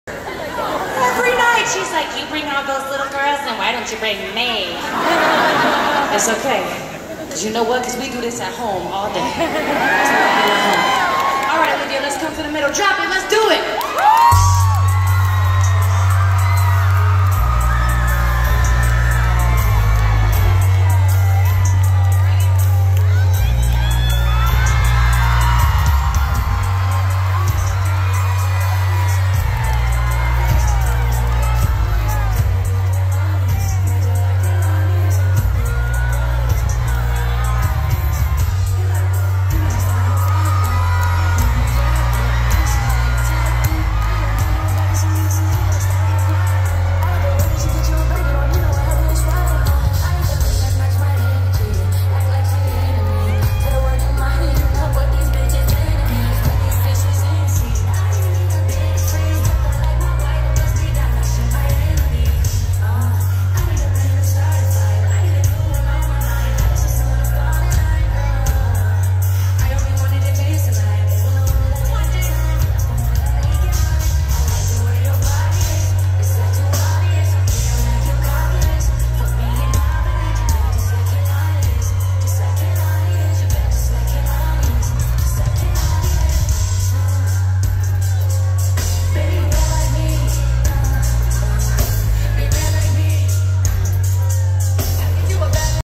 kehlani dancing
@kehlani concert and my daughter Olivia on stage